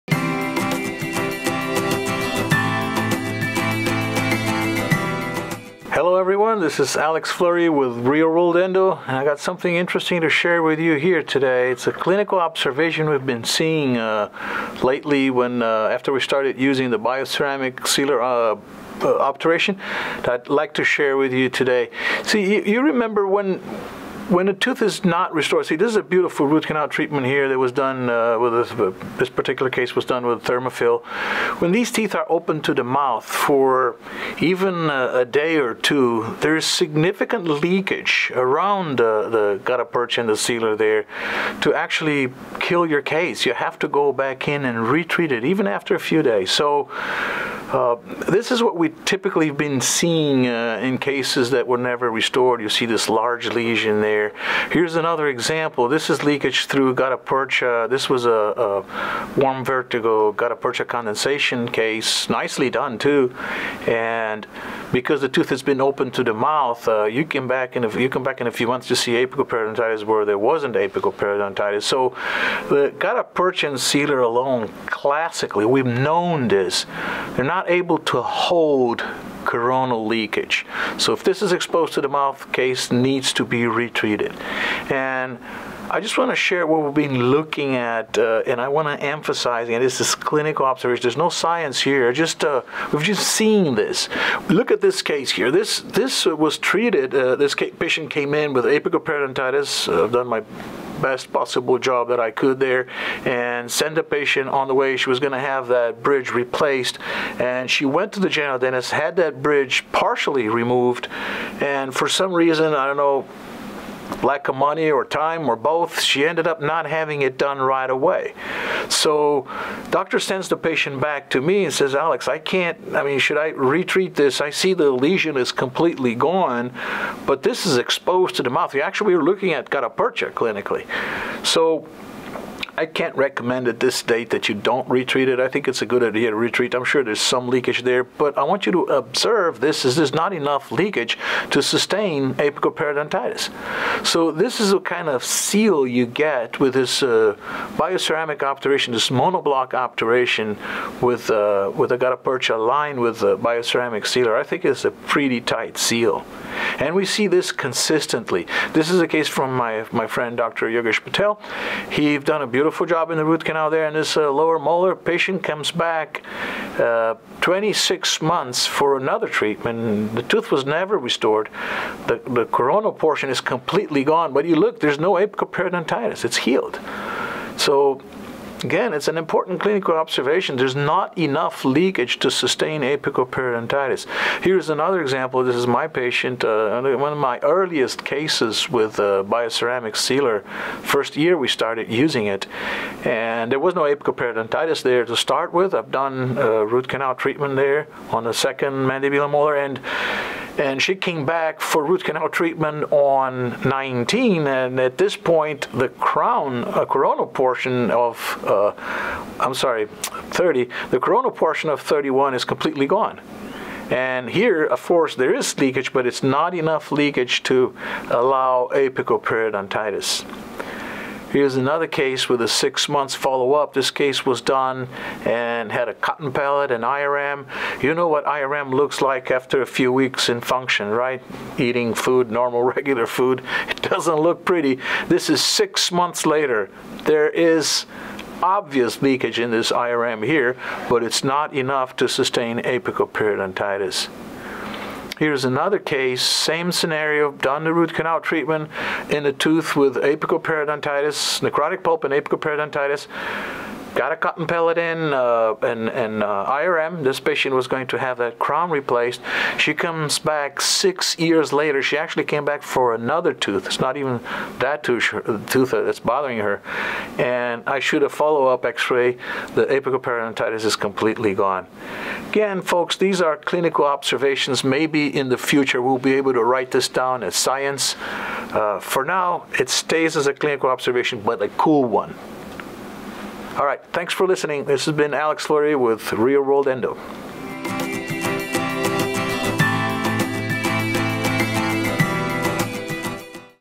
Coronal Leakage Prevention with Bonded Obturation? (Dr. Alex Fleury Podcast)
Dr. Alex Fleury talks about his experience with bioceramic obturation and it's possible effect on coronal leakage prevention.